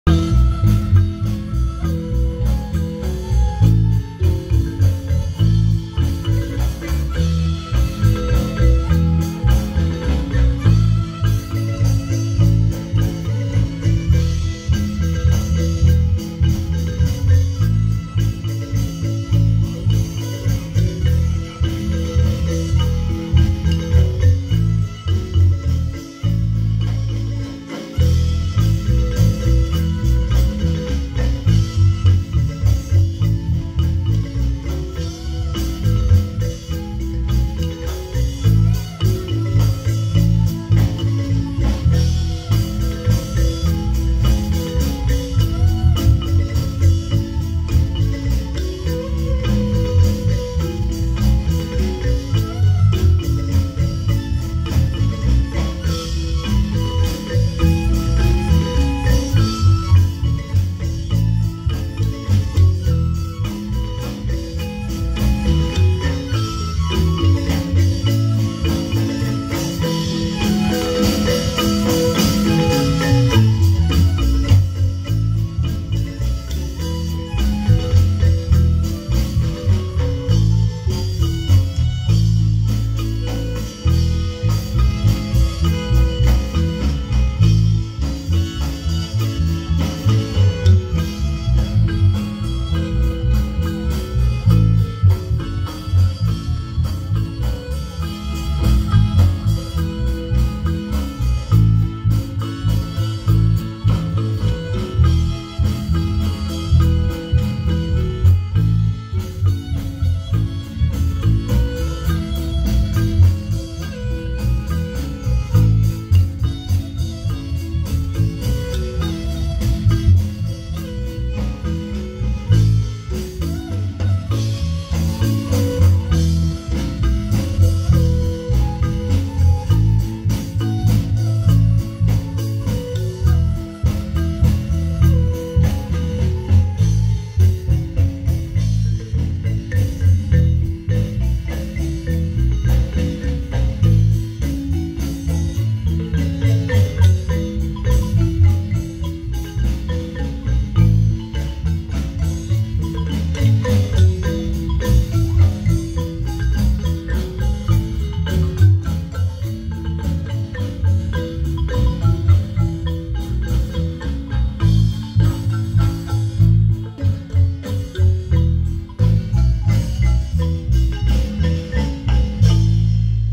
1. Fiesta Titular de San Rafael en Los Angeles | Marimba Primaveral de Guatemala
Fiesta Titular de San Rafael la Independecia en Los Angeles, California 2019